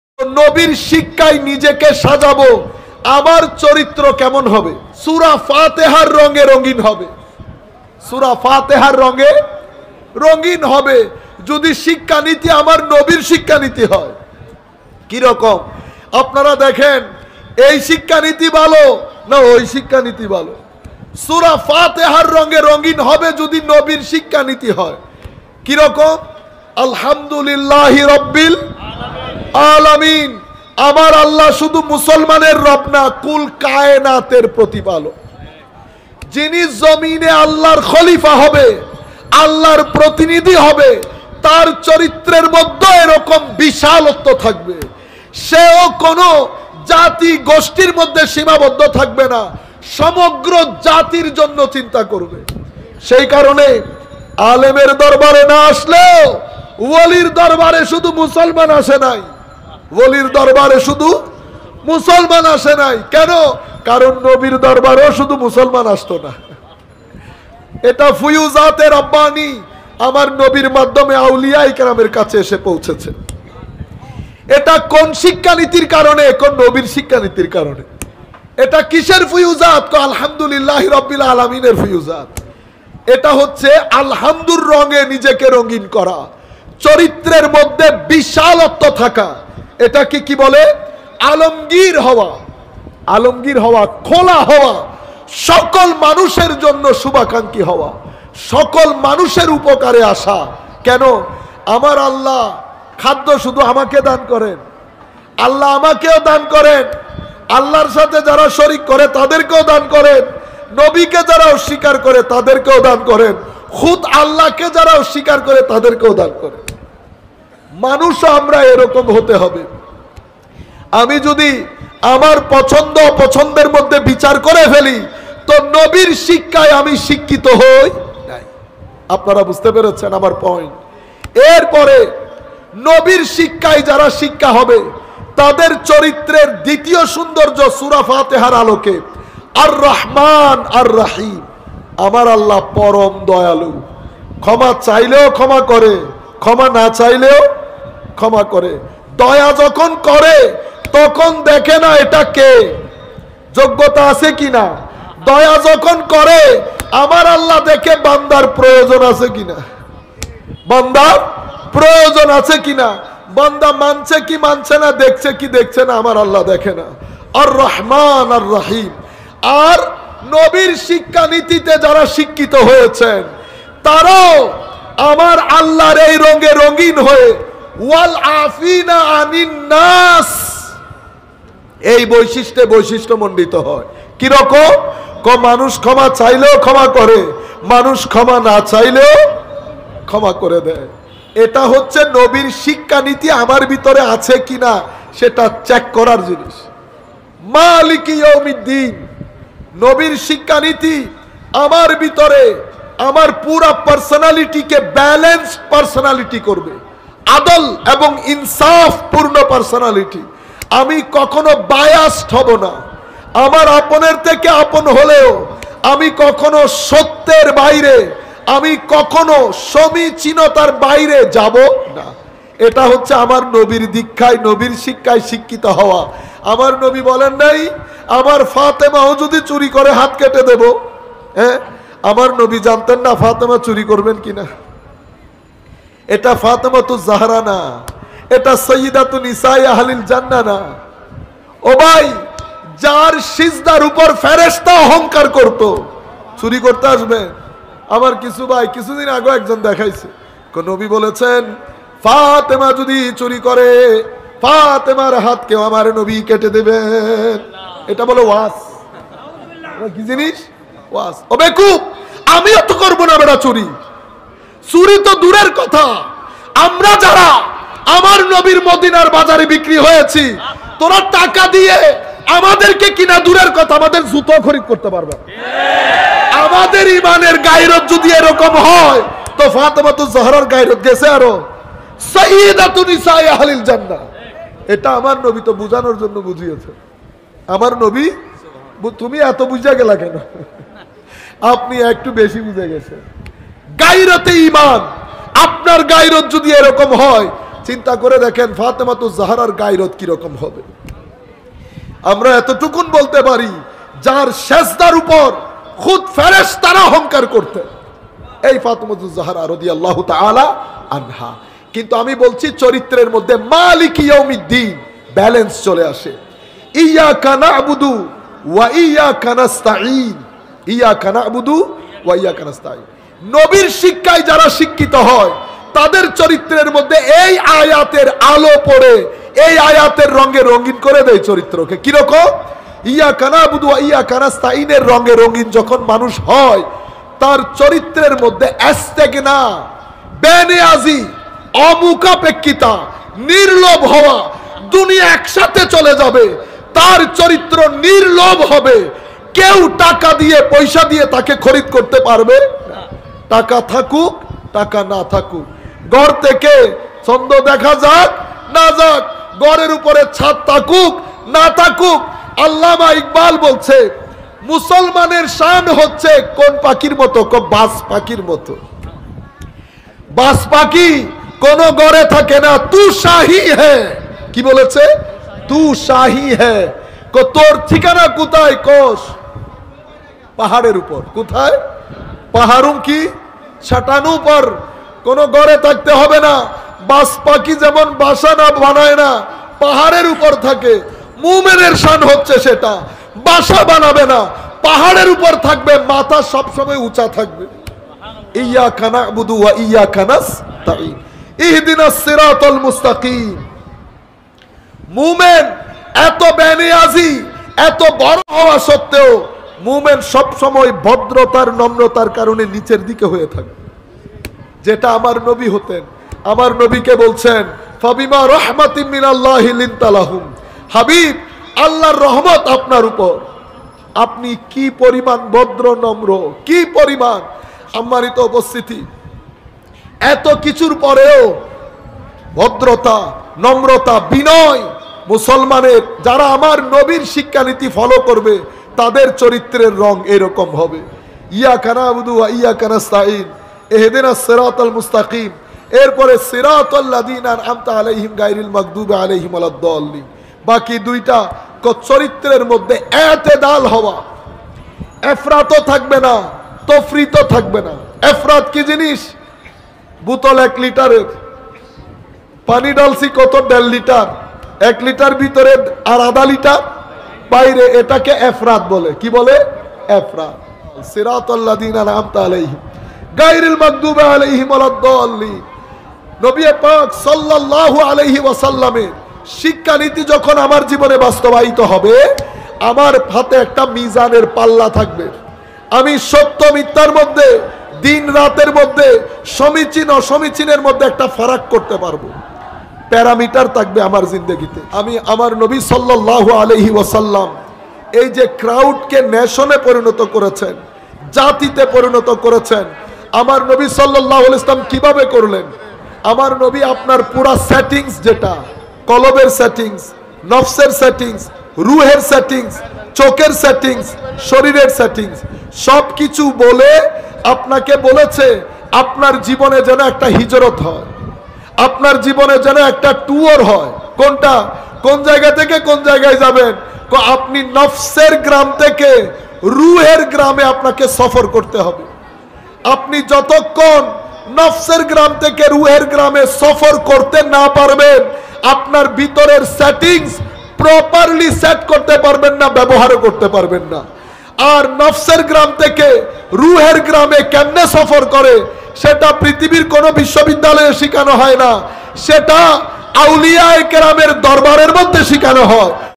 নবীর শিক্ষা নিয়ে সূরা ফাতিহার তাফসীর শায়খ সাইফুল আজম আজহারী | Bangla Waz | Nasimia Tv
Surah Fatihar is a commentary on the description of what character will be like if one acquires the teachings of the Prophet.  In this video, in addition to Surah Fatihar Tafseer, if the Prophet's teachings are added to his character, what form the character will take in the future is understood with Surah Fatihar Tafseer.  If you implement the mentioned things in your life, the character will be like the character described in Surah Fatiha, which will help your life to be beautiful.